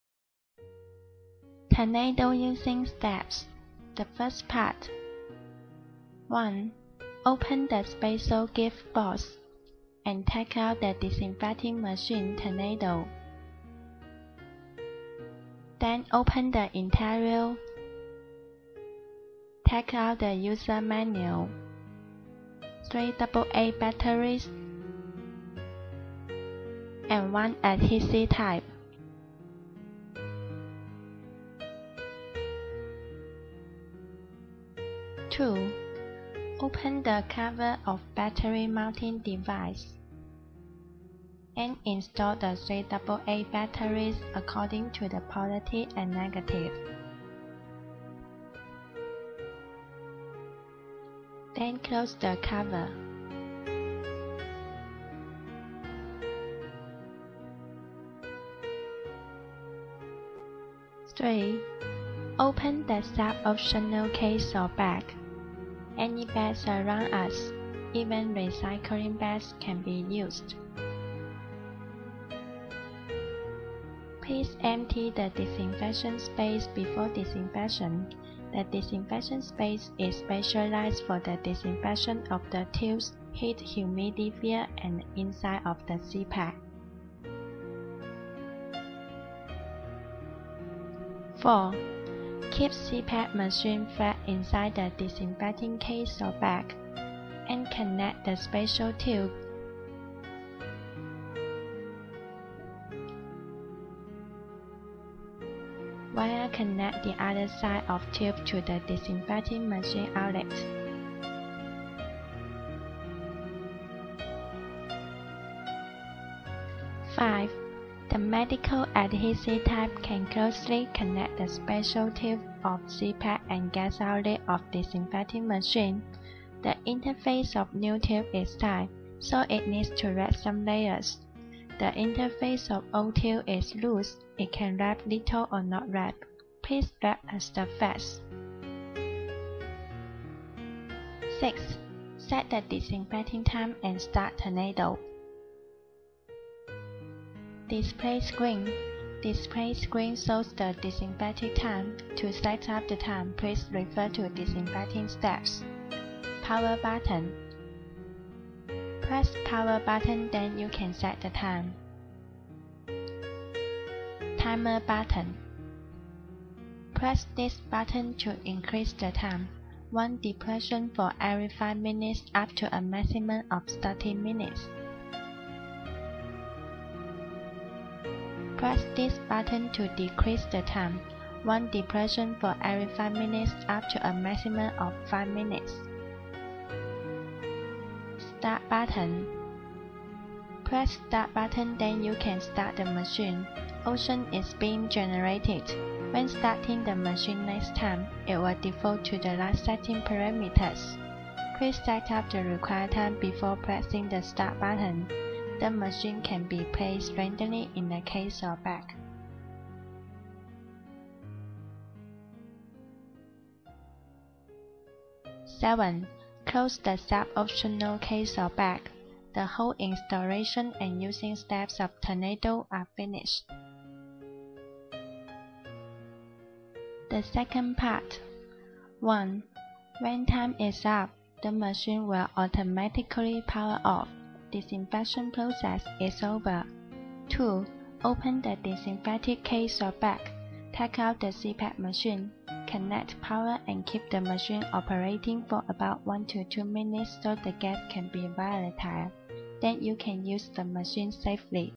CPAP Disinfector and Cleaning Device: Tornado by CPAPGuardian
The new generation of CPAP Guardian--a revolutionary design. It is closer to user needs, more obedient to customer habits.
More powerful function
     ☉Tornado can disinfect the tube and the heated humidifier which can not be cleaned easily.
     ☉Tornado automatically releases disinfecting gas to permeate thoroughly the interior of   CPAP.
     ☉Tornado increases the output of plasma, while ozone is the same. The mixed gas is effective to break downsubstances or sensibiligen, such as pollen, formaldehyde, Benzene, methylbenzene, ammonia, sulfur dioxide, radiation, Co, Co2, ect.

Simpler operation
     ☉Tornado uses double A battery, so it can disinfect everytime and everywhere.
     ☉While disinfecting, it do not need to start the CPAP and not need alternating current and simulative blocking cup.
     ☉It don't need a special bag, the disinfecting operation is easier.

Economical and practical
    ☉Any bags around us, even recycling bags can be used for the disinfecting space. (The size of bag is not more than the suggestion.)
    ☉It can use rechargeable batteries which are economical and environmental protection.
    ☉Because it don't need a special bag, the costs of manufacture and maintenance decline considerably.
    ☉It do not need to start the CPAP, so it can avoid wasting the machine.